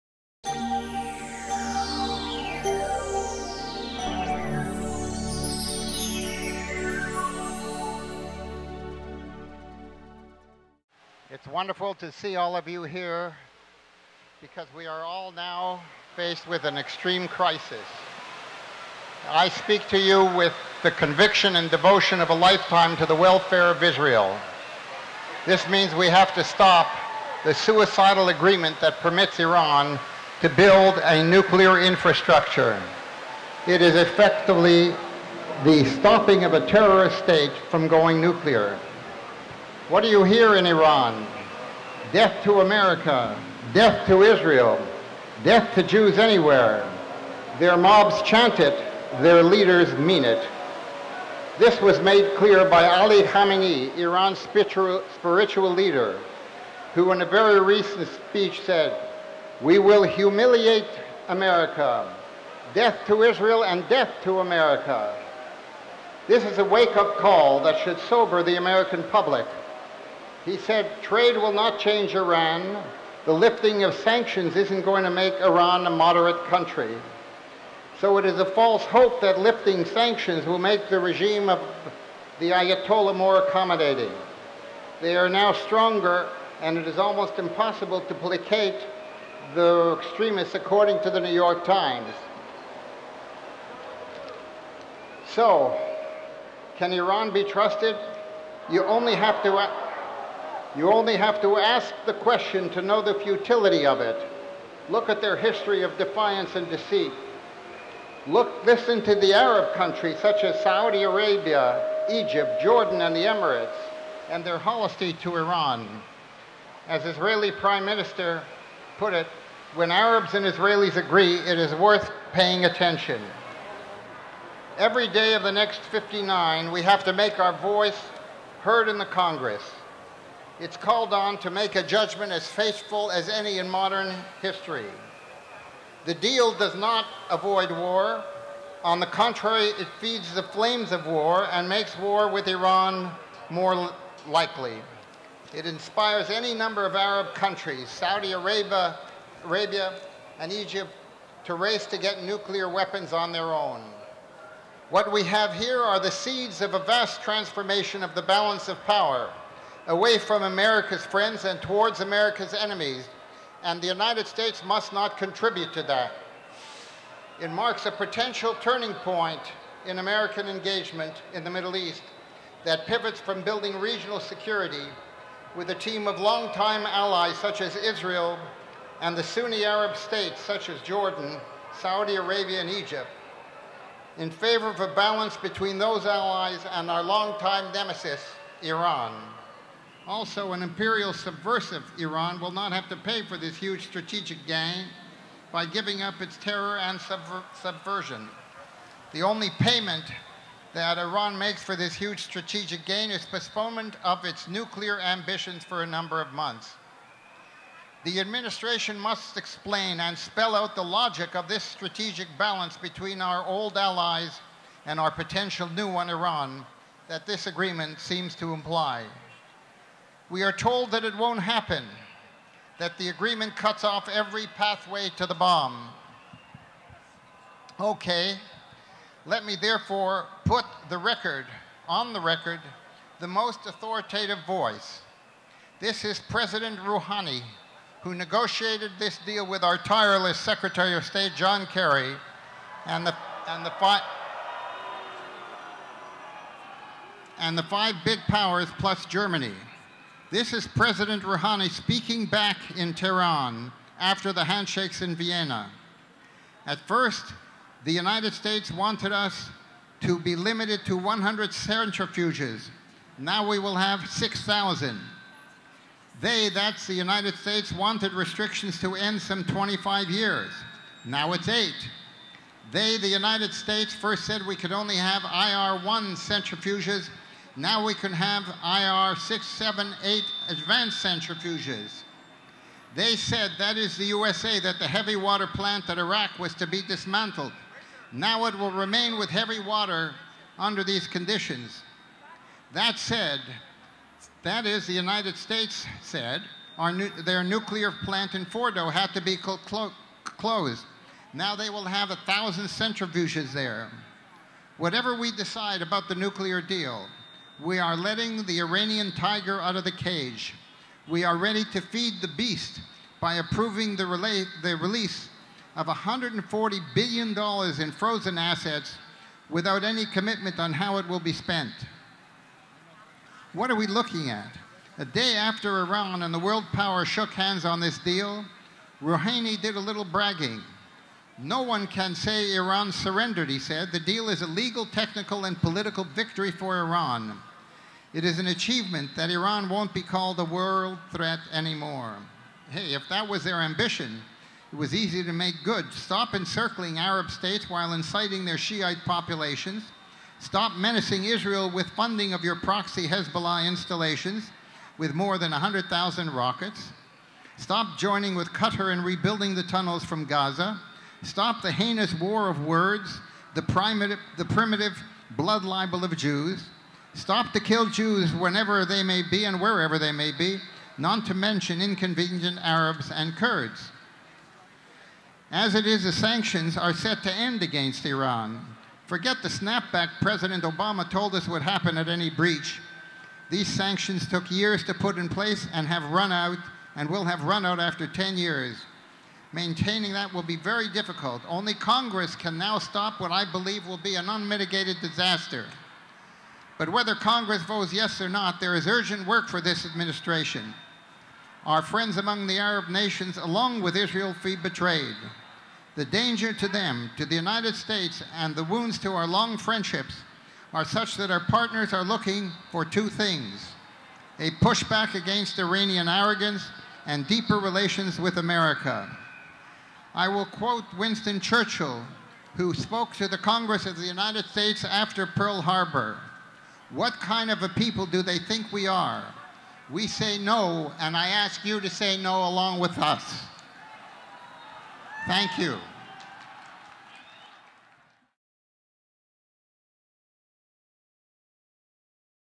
Enlace Judío - 'Stop Iran Now' Rally - Mortimer Zuckerman - SUBTITULADO
El pasado 22 de julio se llevó a cabo en Times Square, en la Ciudad de Nueva York, el rally (mitin) Stop Iran Now (Detengan a Irán ahora), al que asistieron más de 10,000 personas para escuchar, en la voz de varios congresistas, senadores, miembros del Ejército estadounidense y diversas personalidades de la política norteamericana, que su Presidente Barack Obama ha cometido un grave error y que el acuerdo nuclear con Irán es peligrosísimo. Todos ellos están a favor de que se nulifique o ‘eche para atrás’ dicho acuerdo.